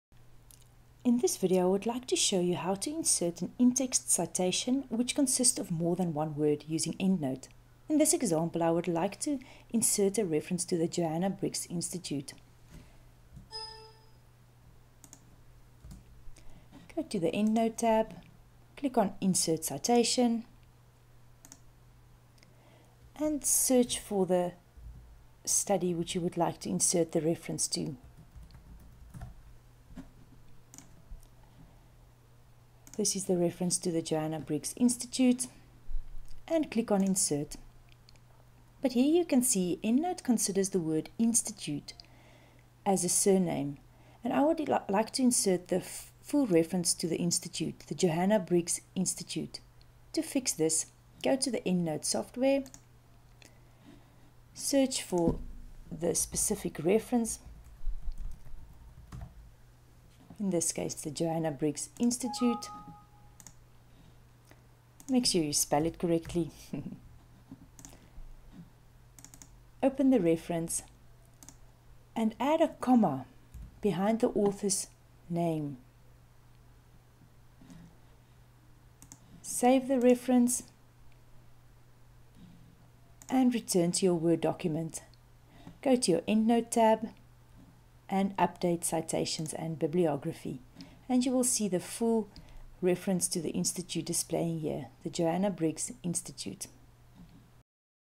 Endnote citation: How to insert an in-text citation consisting of more than one word using Endnote
This video will show you how to insert an in-text citation which consist of more than one word using Endnote e.g. (Word Health Organisation, 2016) instead of (Organisation, 2016)

Here's a direct link to download the Endnote software